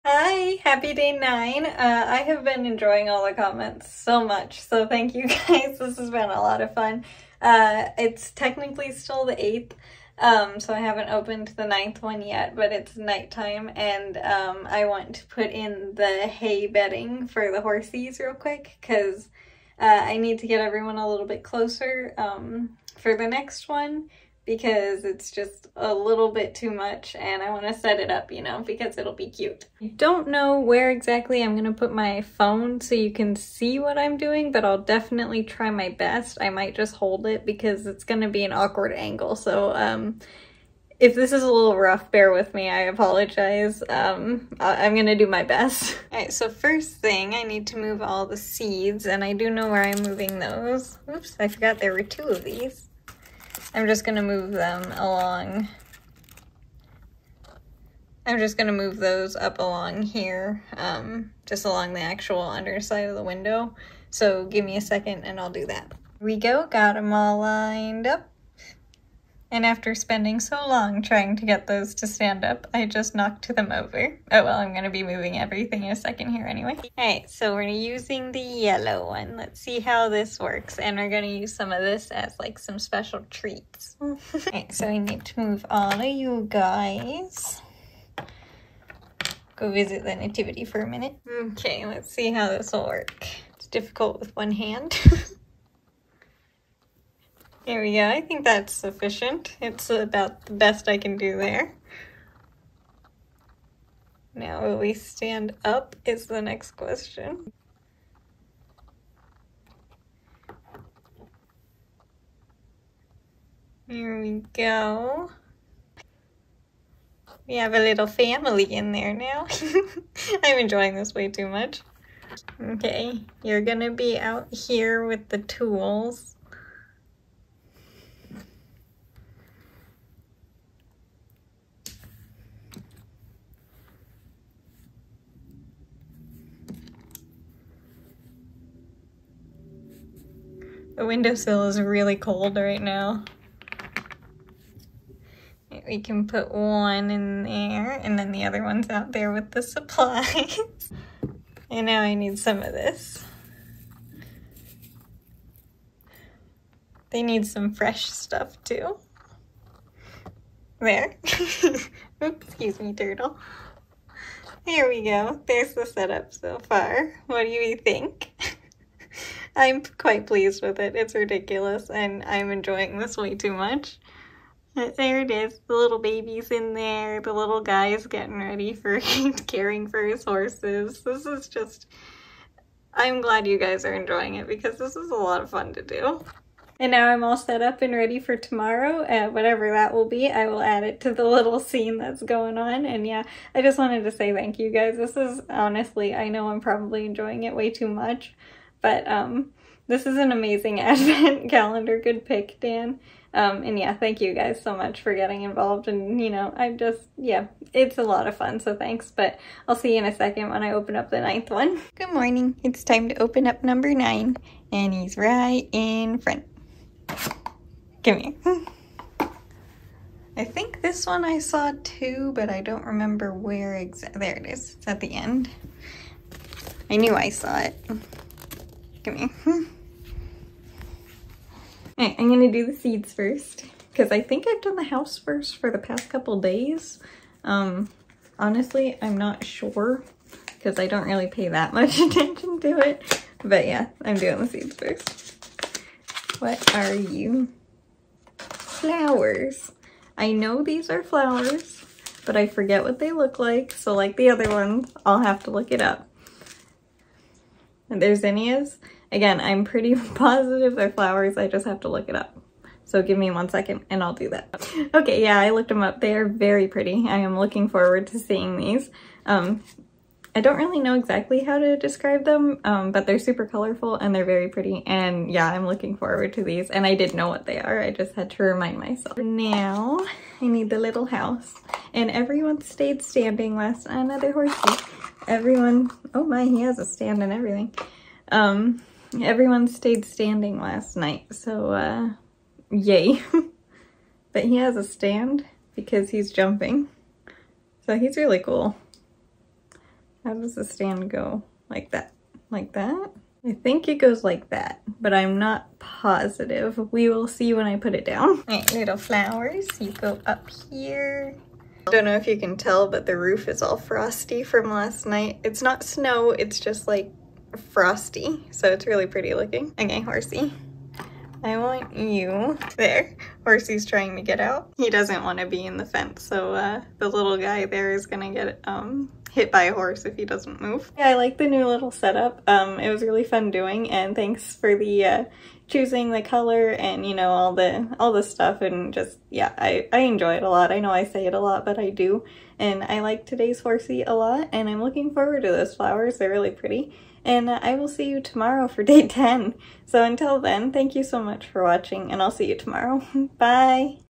It’s the 9th advent day!! And making the set more put together 🤍
lol I’m having way too much fun with this. I’ll admit it :). But I love reading your comments and how much you guys are enjoying it too :). It’s just a lot of fun :). So thank you!! 🤍 but today I decided to do a little rearranging of everyone :). Create more of a scene lol. Meant to say lemme know if you want anyone moved around.. but forgot sooo ya oops :) I’ll try and remember tomorrow :). But i think it looks pretty cute. The new horsey wants nothing to do with the family or being in a fence lol. And as for the seeds I did know what they were!! Just had to remind myself :). And I think these are gunna be super pretty out there. All the flowers I’ve gotten are super colorful :). I really enjoy 🤍 and am very glad you’re enjoying it too!! Thank you so much for everything 🤍 I’ll see ya tomorrow!!